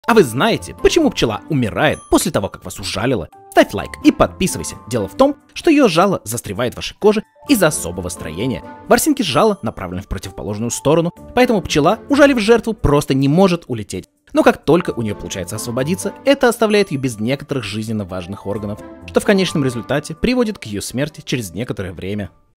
Почему Пчелы Умирают 🐝 (после укуса) ✨
Почему пчелы умирают после укуса? Кто пчелы, осы, шмели или шершни погибают после того как ужалят? Гибнут ли осы после укуса?

Короткие видео факты. Короткие интересные и удивительные факты обо всем.

🎥 zackdfilms